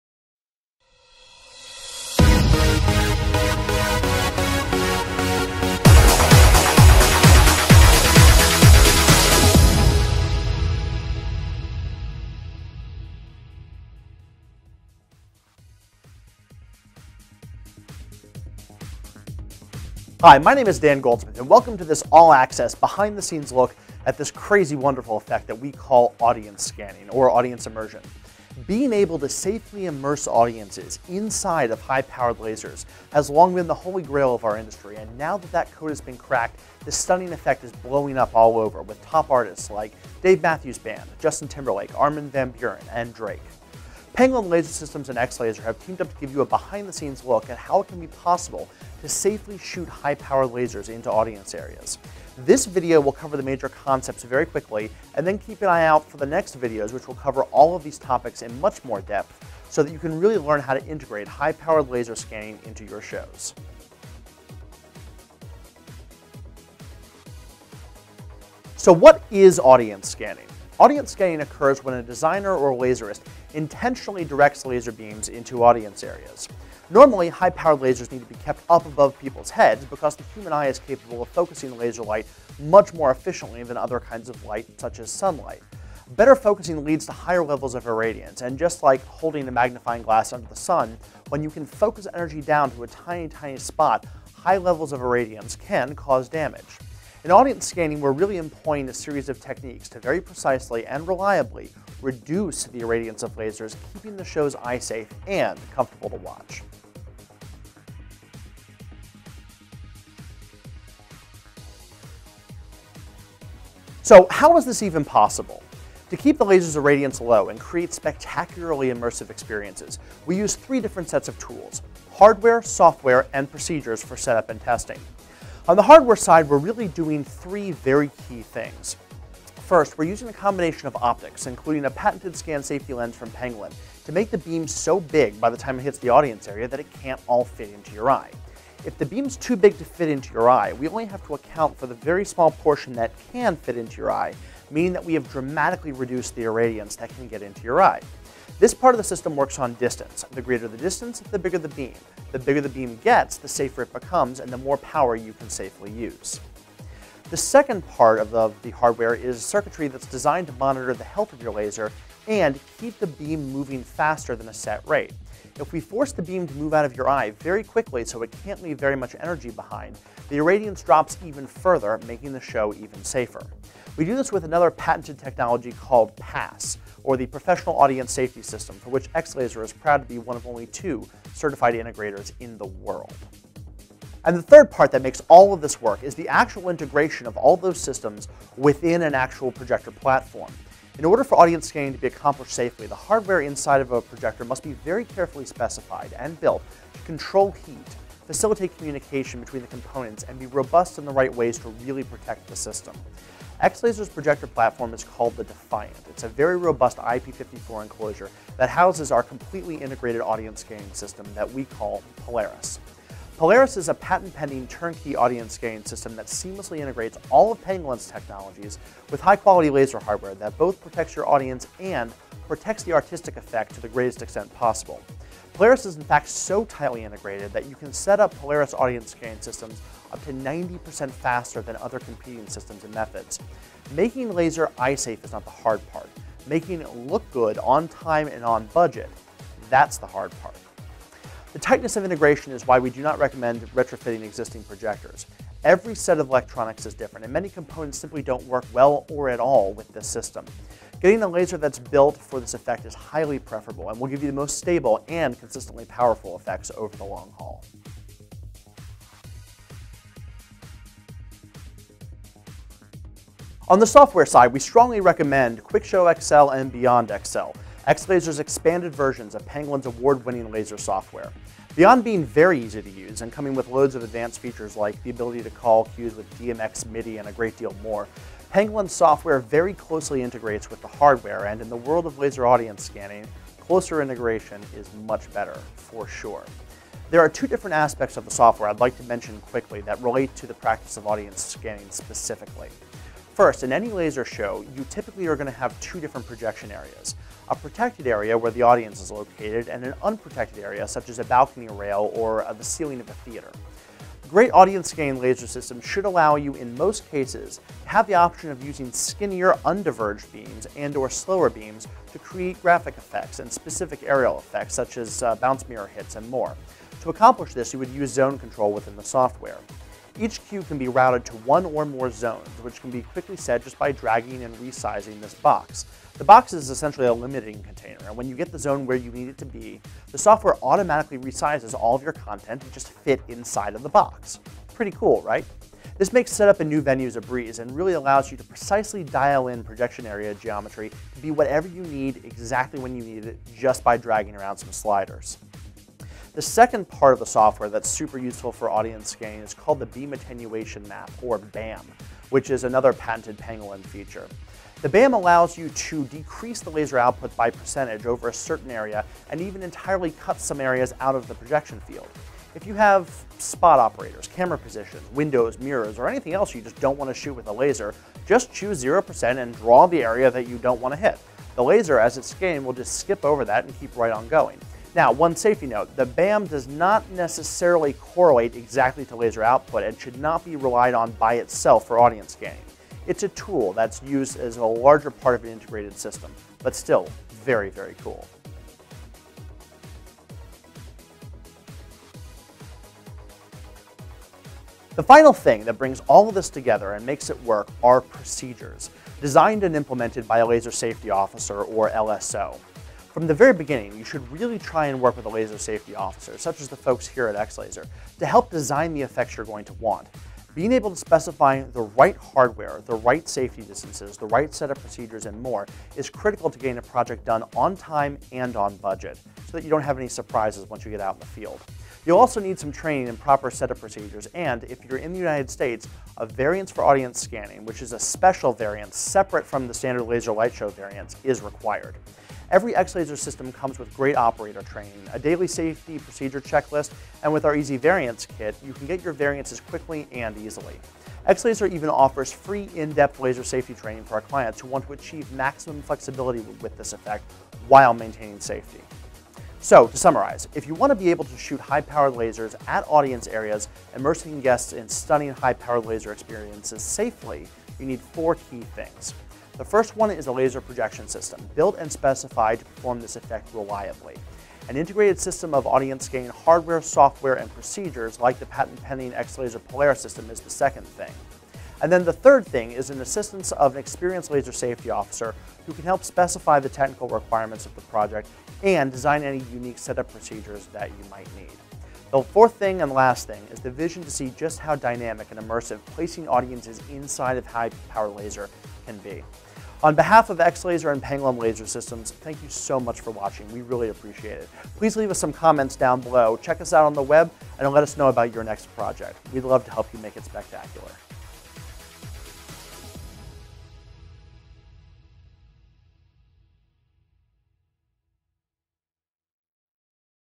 Polaris: A Closer Look at Safe, Legal, Audience Scanning.
Polaris brings the idea of safe, legal, audience scanning to life, by putting the simplest and most affordable FDA compliant audience immersion system into X-Laserʼs industry leading Skywriter and Defiant projector families. Combining X-Laserʼs revolutionary compliance procedures with patented safety technology, Polaris can be scaled to nearly any size venue -- quickly and easily. Polaris makes everything possible, see why and experience the true power of Polaris.